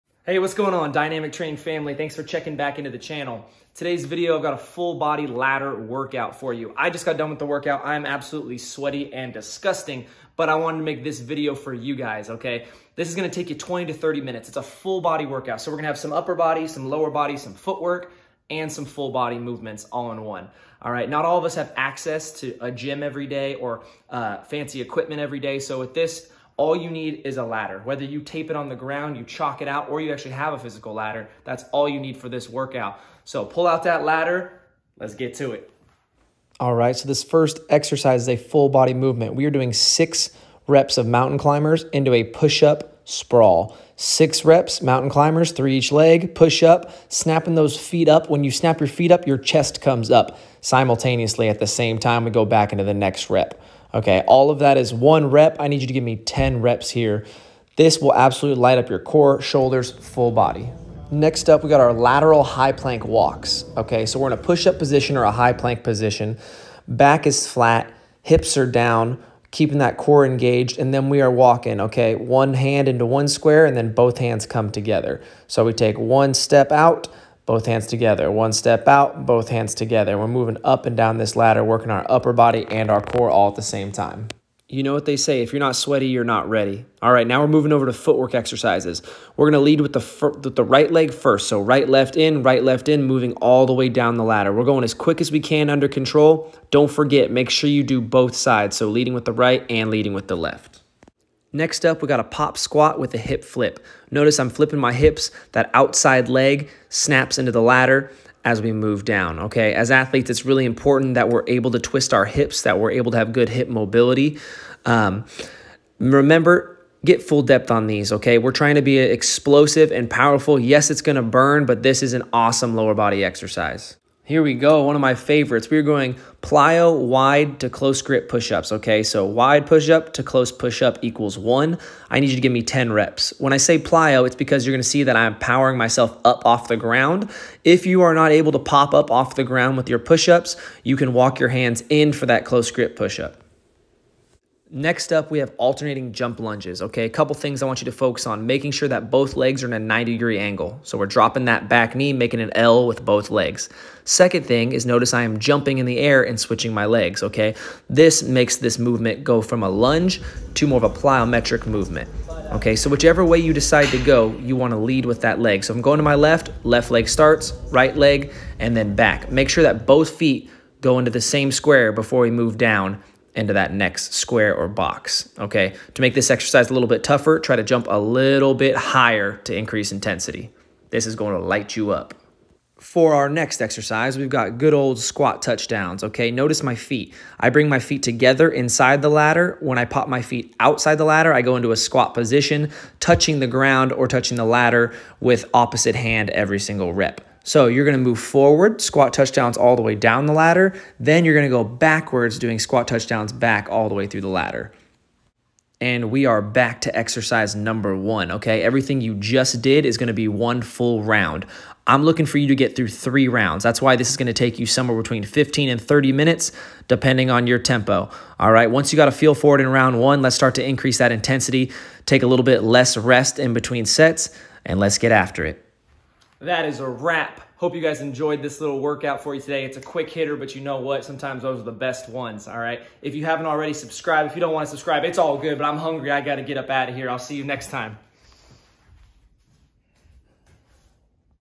20 Min Full Body Ladder Workout
Full￼ body ladder workout. This workout should take you about 20 to 25 minutes depending on your pace. This full body workout includes some foot work some upper body some lower body and some full body movements. If you’re looking for a work out to do at home over the weekend or something quick to add to what you were already doing this is a great work out for you. It doesn’t matter if you’re looking to increase your cardio looking to lose weight or looking to improve athletic performance. This full body ladder work out attacks every area.￼￼